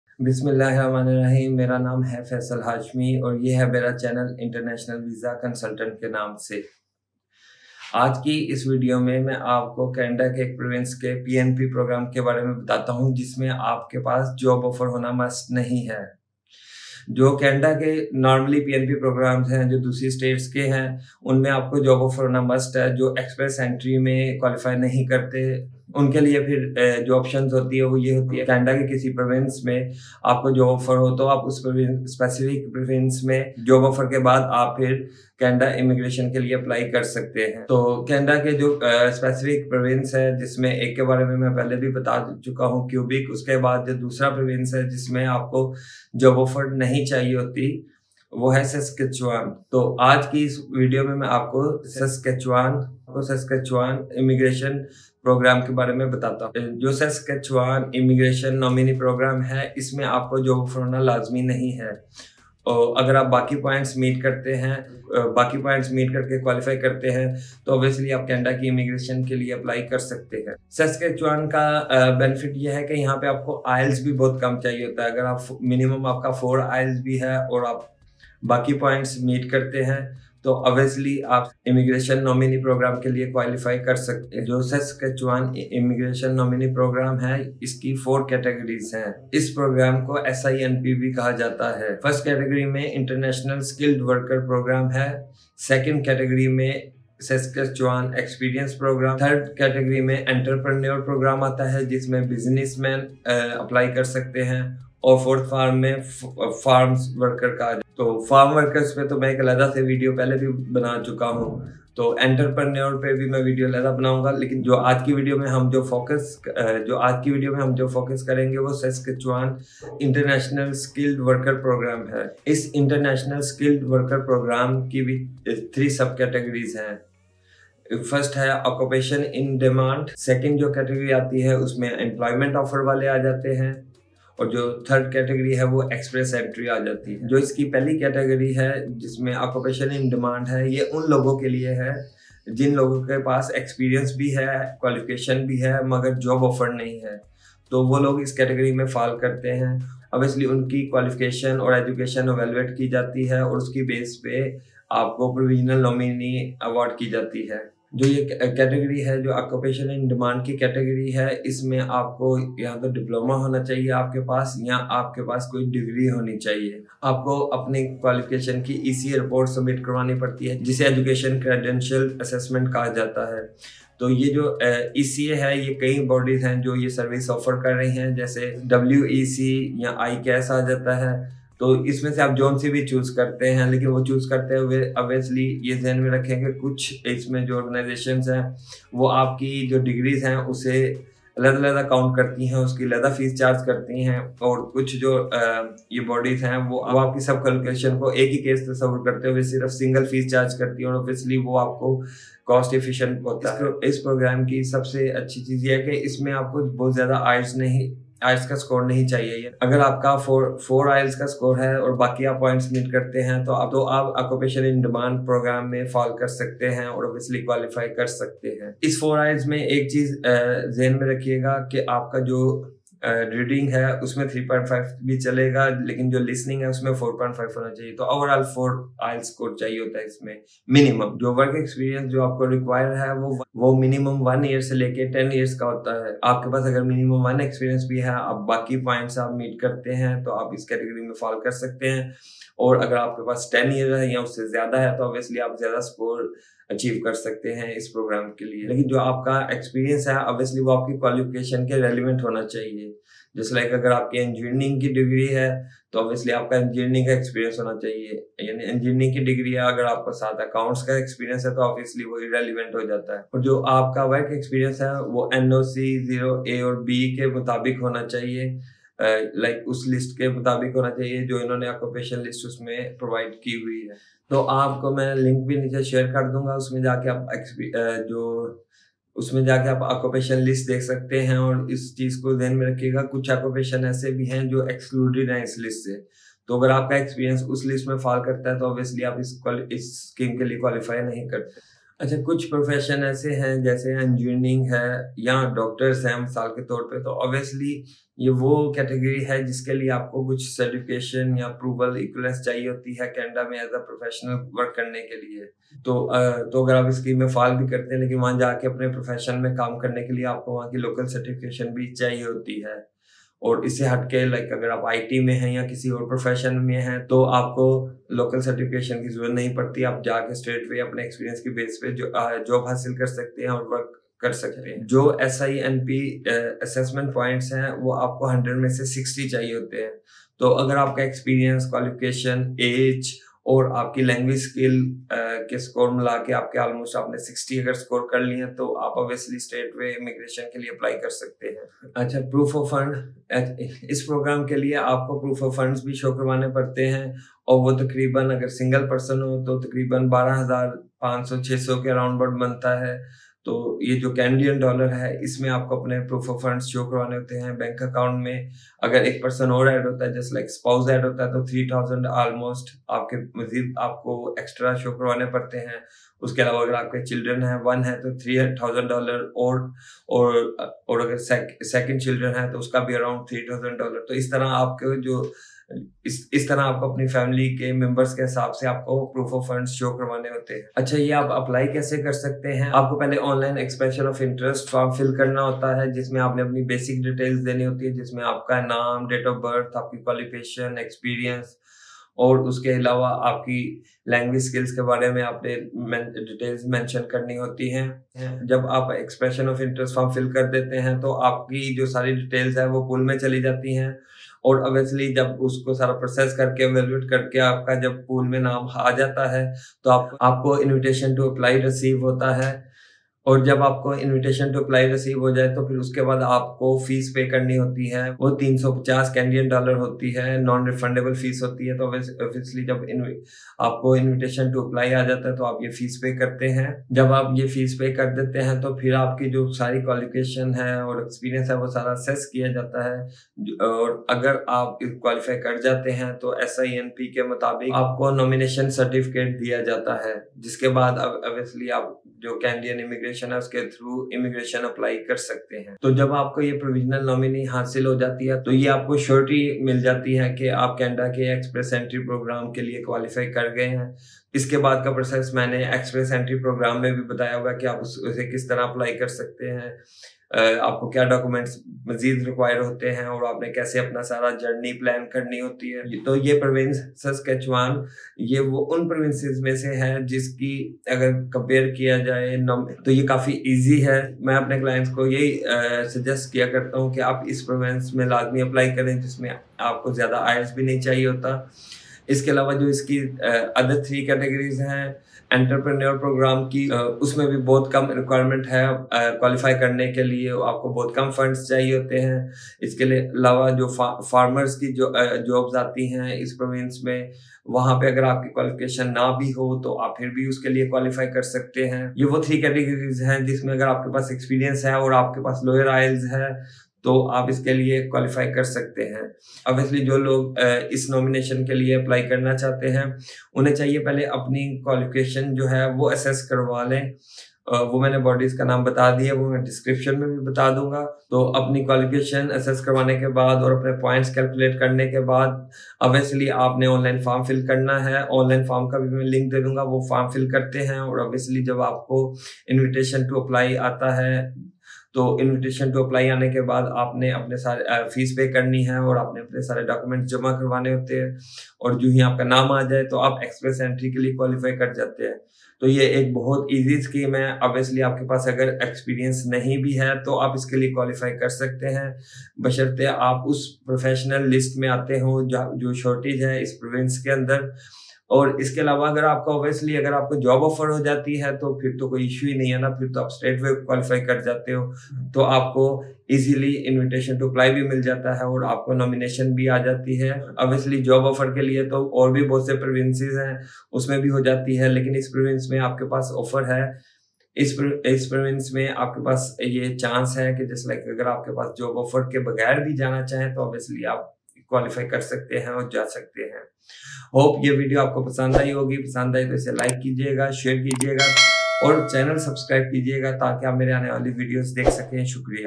Canada Immigration Without Job Offer || Saskatchewan Nominee Immigrant Program (SINP)
Canada Immigration Without Job Offer-SNIP---Saskatchewan Nominee Immigrant Program
Saskatchewan International Skilled Worker Program.